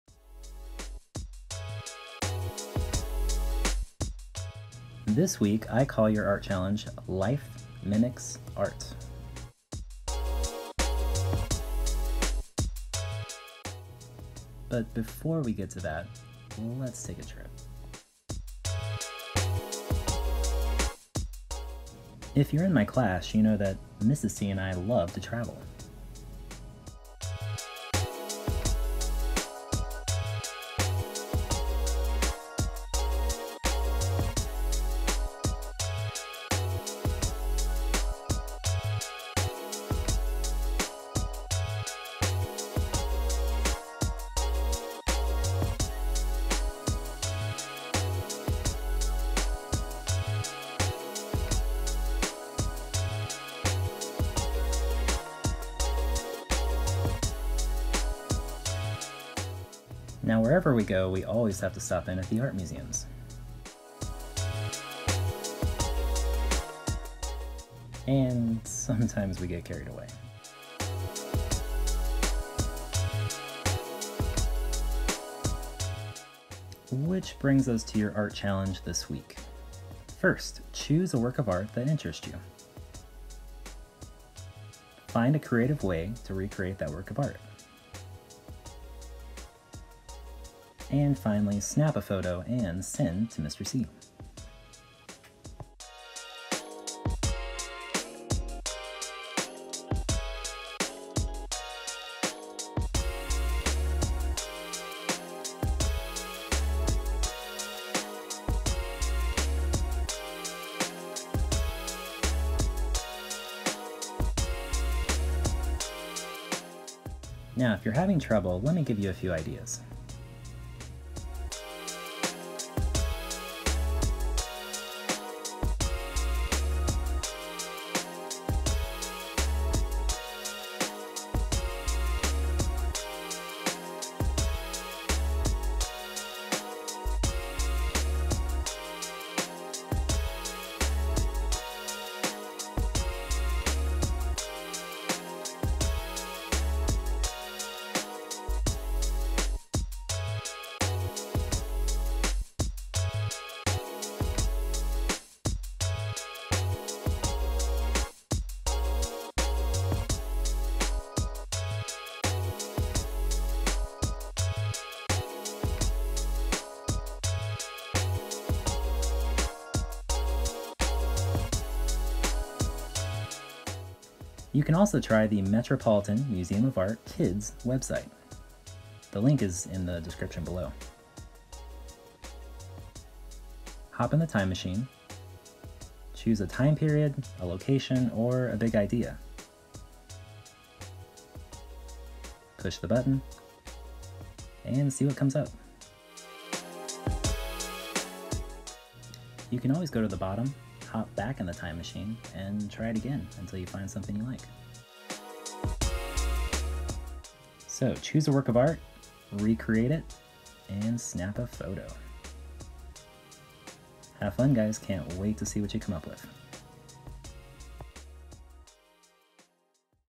Life Mimics Art - Getty Museum Challenge - Easy Art Lesson
There’s a saying that goes “art imitates (or mimics) life” - but this art challenge will flip that around.

Do some research and find a work of art that is interesting to you. Find a creative way to mimic the art you have chosen. And of course snap and photo and share 📷
Get creative and have some fun!